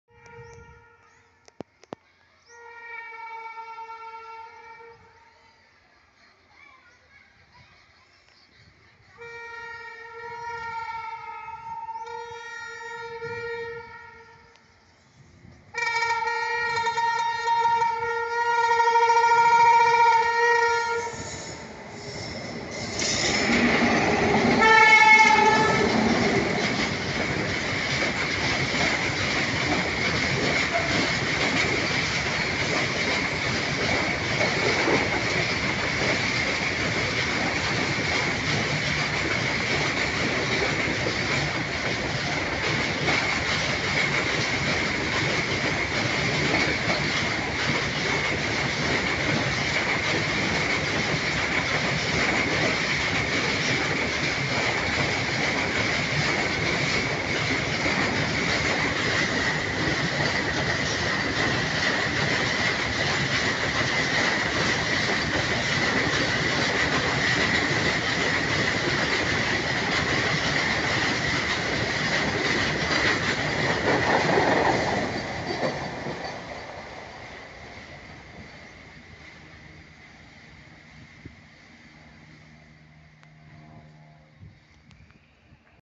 WAG-9 TWINS HONKING||VISAKHAPATNAM LOCO SHED||VSKP WAG9
hello all, welcome to my channel. this video is about honking of WAG9 twins, The locomotive is pushing the goods train as it's pantograph is reverse, they belong to Visakhapatnam loco shed.The honking of the train is a little variety but it is Melodious. please like share and subscribe for more videos